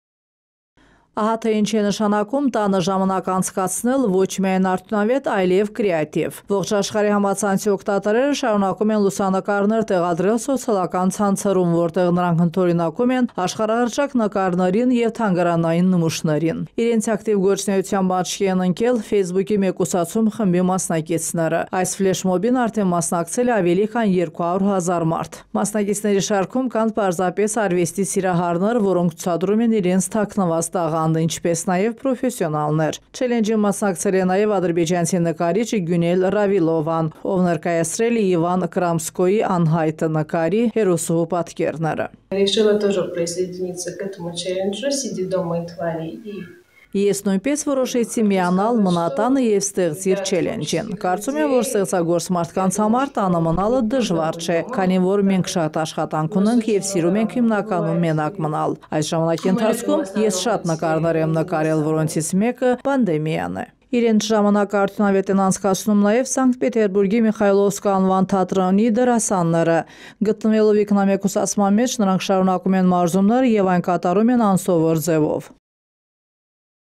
Ստեղծագործել ինքնամեկուսացման պայմաններում
Ահա թե  ինչ է  նշանակում տանը ժամանակ անցկացնել՝ ոչ միայն արդյունավետ, այլև կրեատիվ:  Ողջ աշխարհի համացանցի օգտատերերը շարունակում են լուսանկարներ տեղադրել սոցիալական ցանցերում, որտեղ նրանք ընդօրինակում են աշխարհահռչակ նկարներին ու թանգարանային նմուշներին: